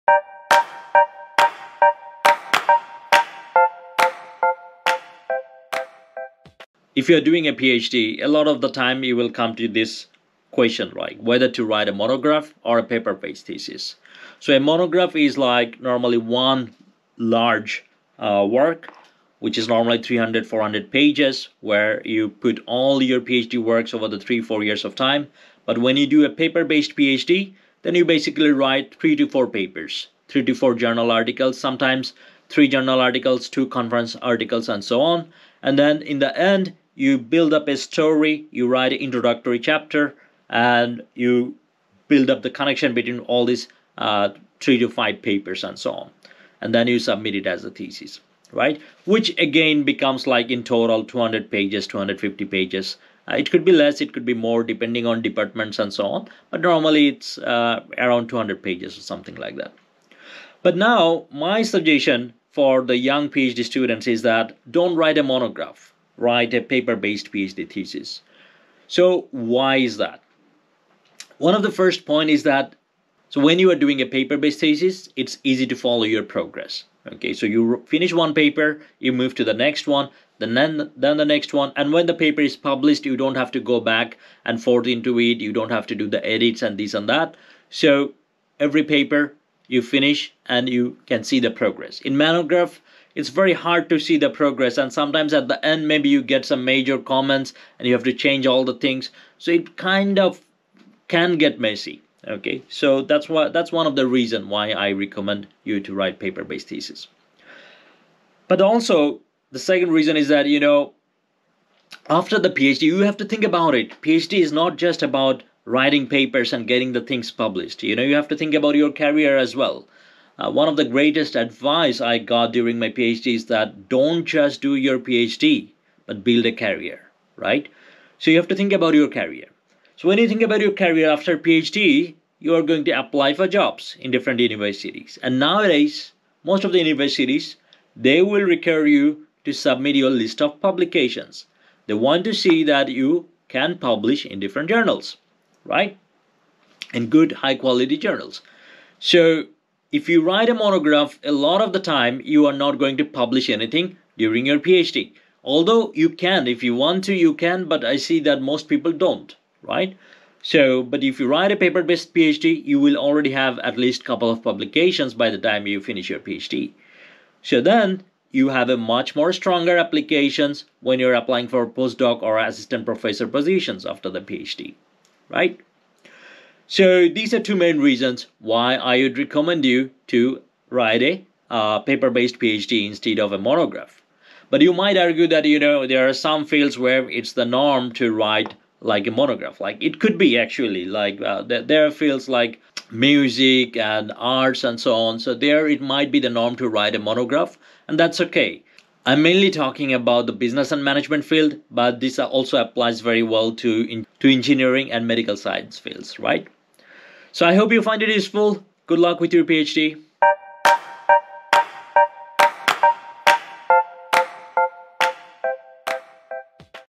Monograph Versus Paper-based PhD Dissertation
Professor Ziaul H. Munim shares his advice on why PhD students should choose paper-based thesis. Content of the video are his own opinion only.